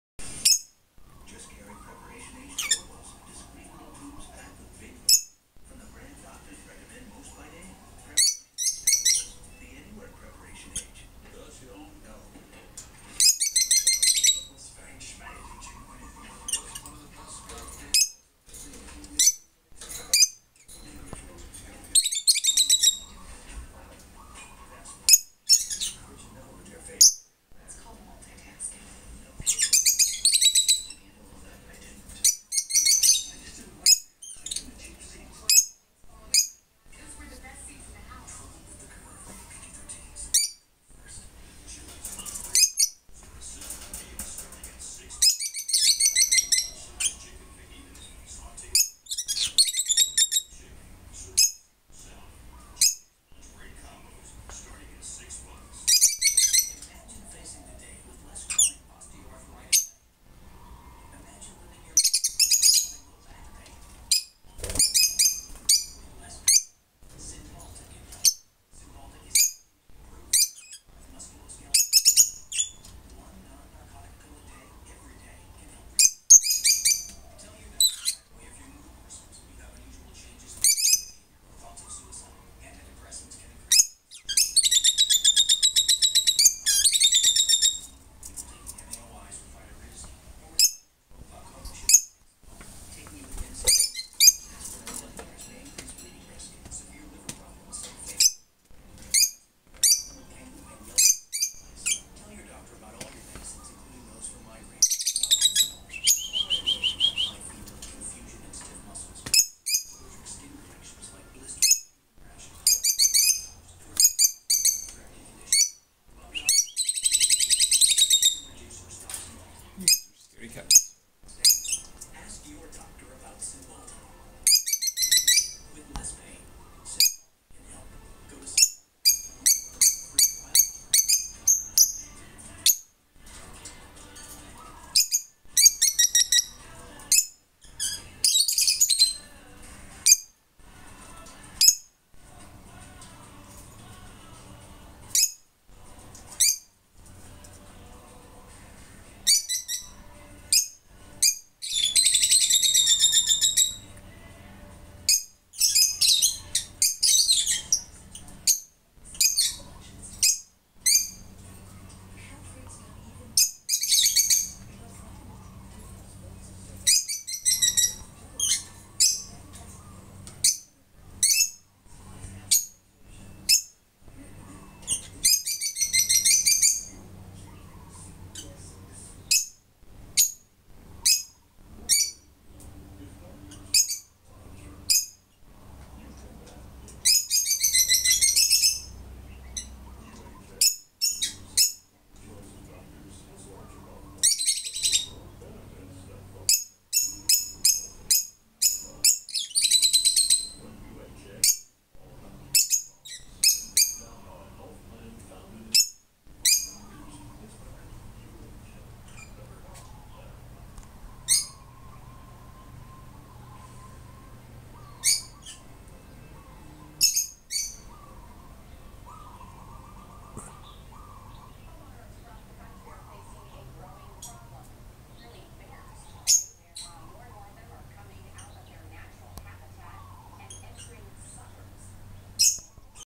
My back yard
This is a little sample of my shangra la!  sans my little squeaky love birds.